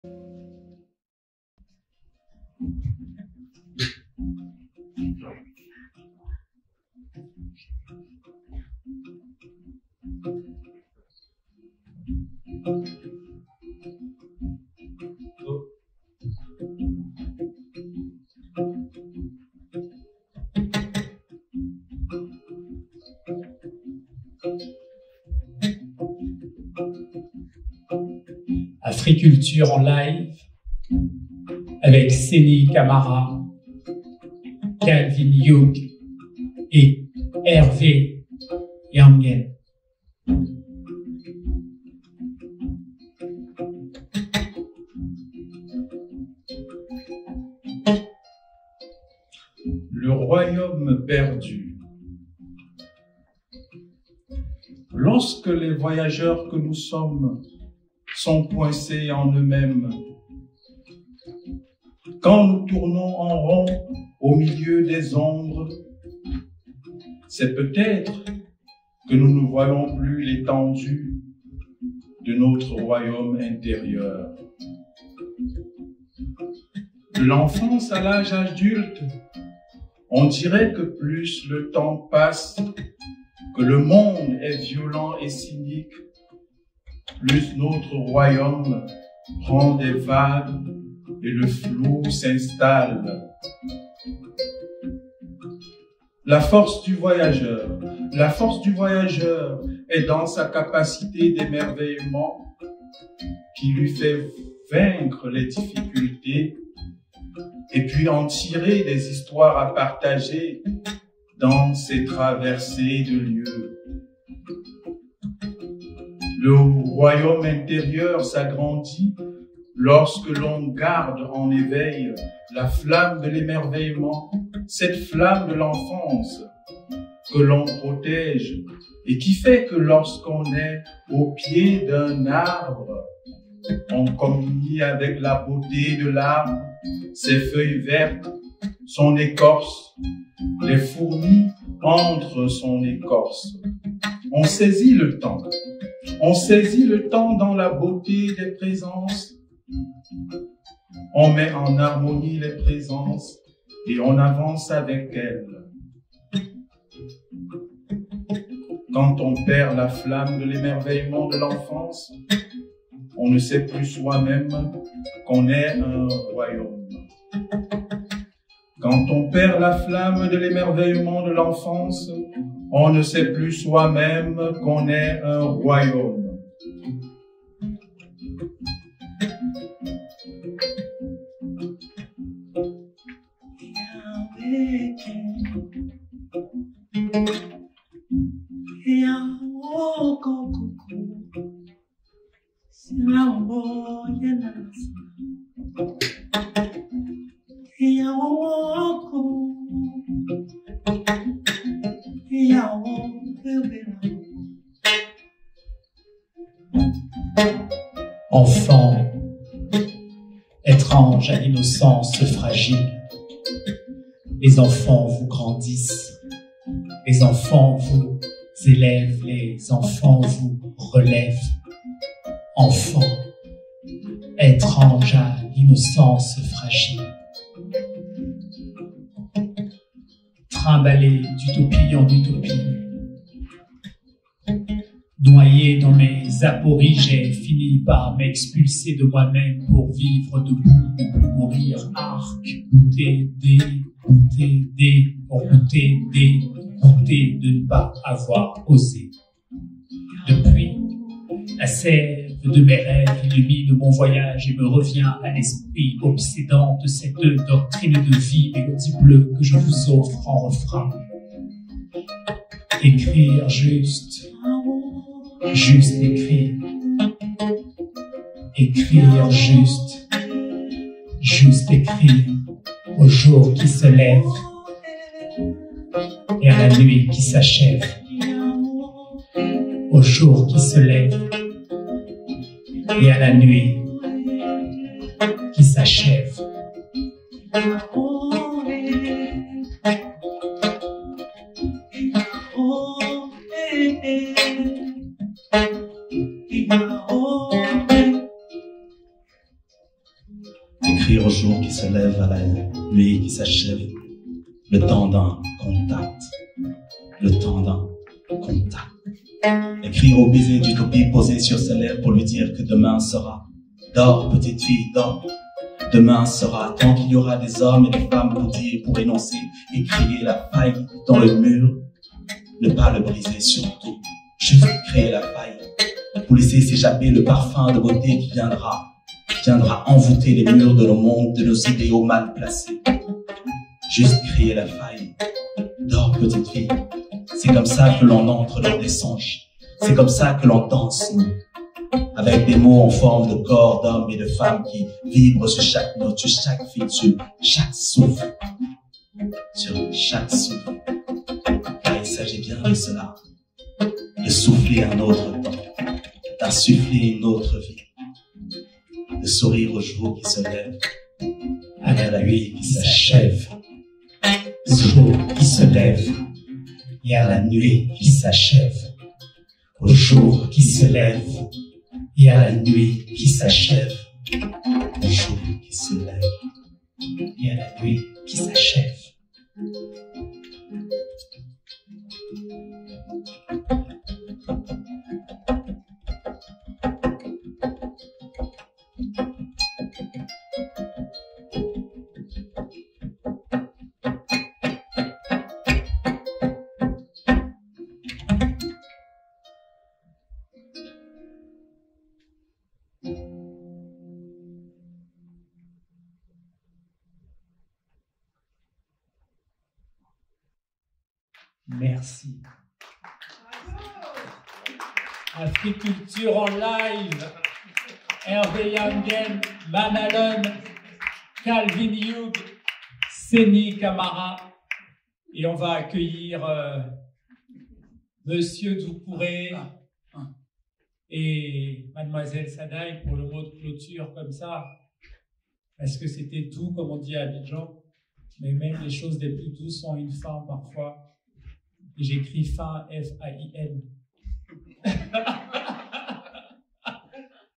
Africultures en live - Final
Final de l'événement Africultures en Live du 7 avril 2021 à la Cité Internationale des Art à Paris.
Marc Alexandre Oho Bambe dit Capitaine Alexandre
Albert Morisseau - Leroy
Senny Camara
Calvin Yug
Réalisation : Maonghe.M